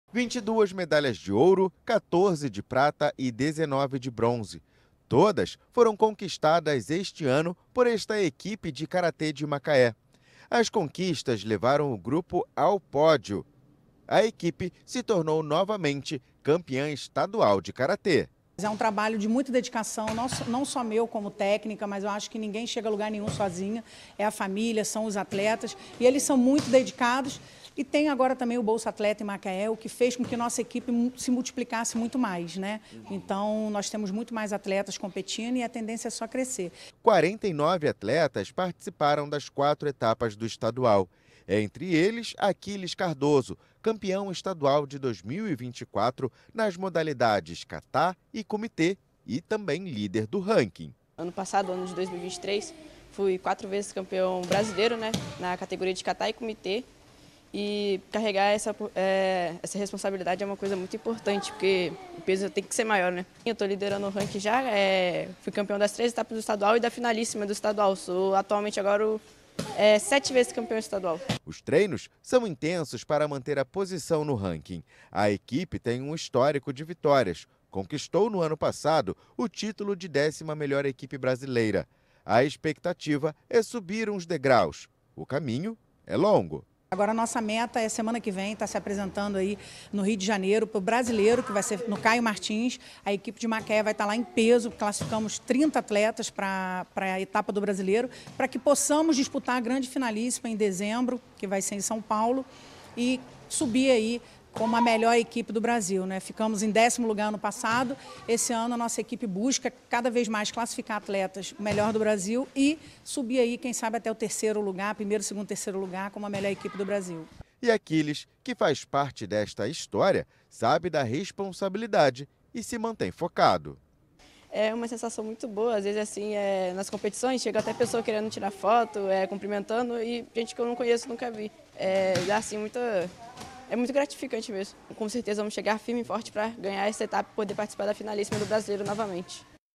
Atletas de karatê de Macaé, brilham no Campeonato Estadual
Em Macaé, atletas de karatê foram campeões do campeonato estadual deste ano e também dos jogos escolares do Rio. O próximo desafio é o campeonato brasileiro, que vai ser disputado no Rio, na próxima semana.